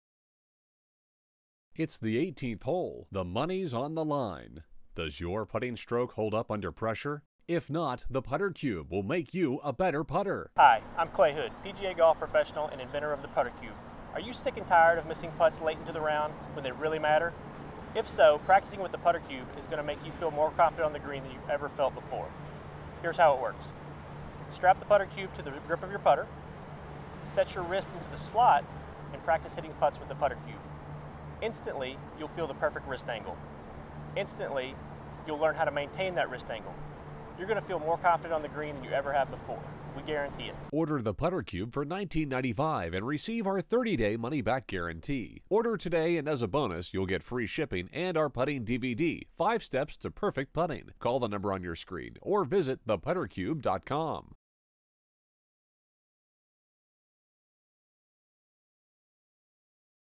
Golf Drills & Golf Tips The Putter Cube | Golf Putting Training Aid | 60 Second Commercial
Get $10 off any Golf Laser Rangefinder: Use Code: 10off The Putter Cube teaches you how to set your wrist at the correct angle and how to maintain that angle throughout the putting stroke.